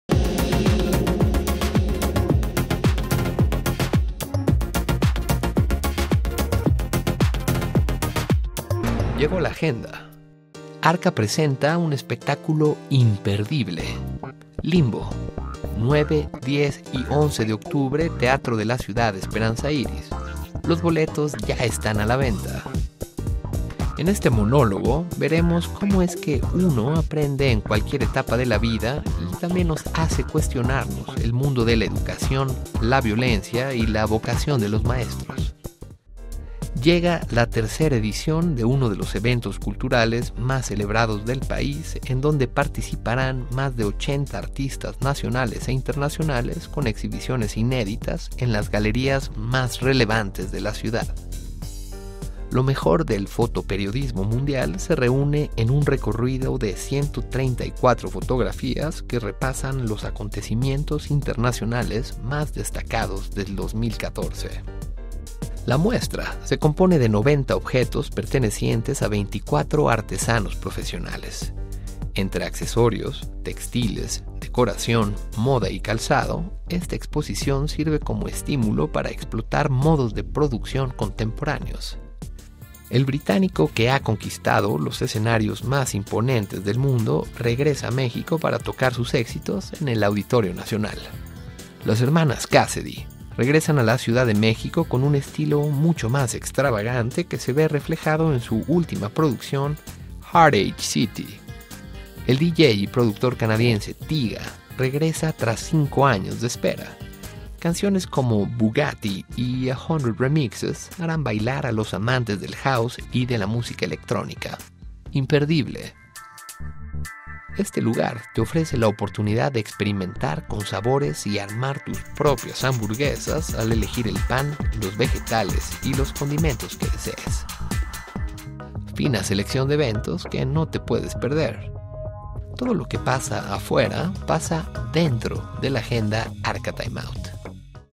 Agenda Arca - Time Out / 16 - 30 Septiembre
¡Llegó la agenda! Arrancamos las celebraciones patrias con las mejores recomendaciones para la segunda quincena del mes patrio.